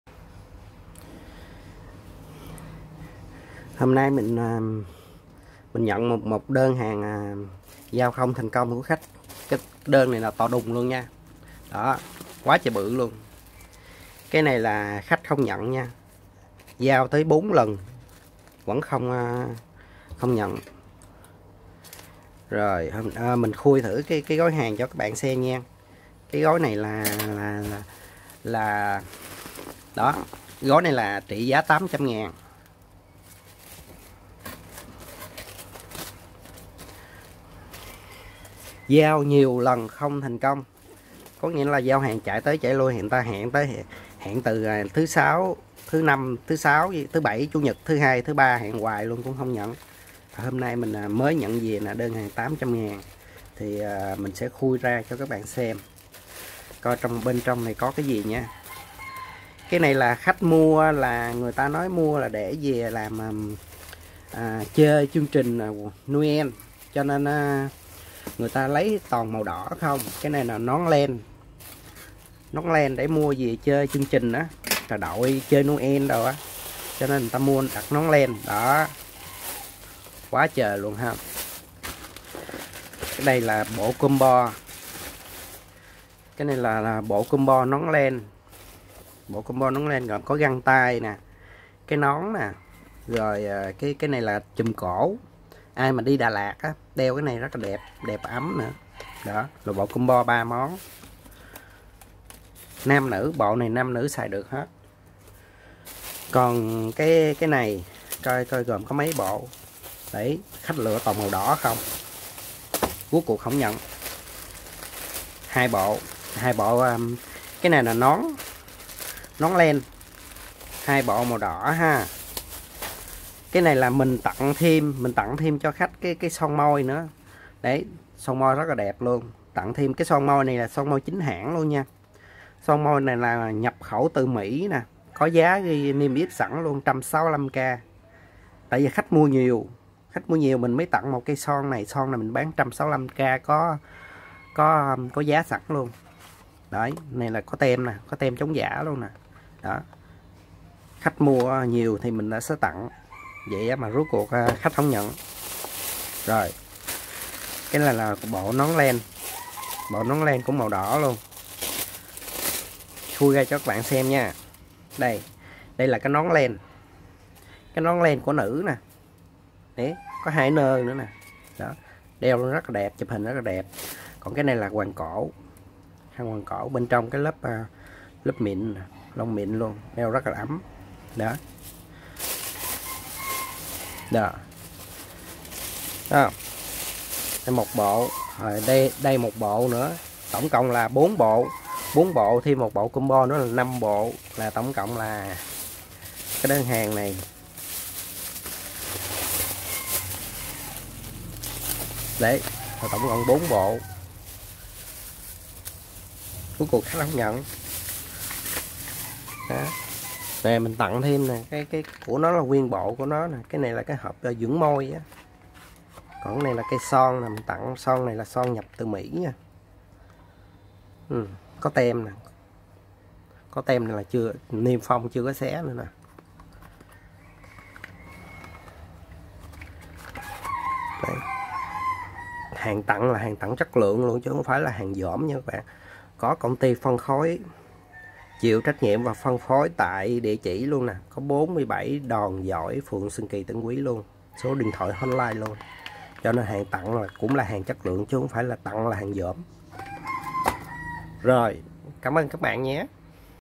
Khui hàng bom khách đặt không lấy
Hôm nay nhận 1 gói hàng chà bá trị trá 800k khui cho các bạn xem chơi
Đơn này khách hẹn ngày này qua ngày kia vẫn không lấy, nên cuối cùng trả về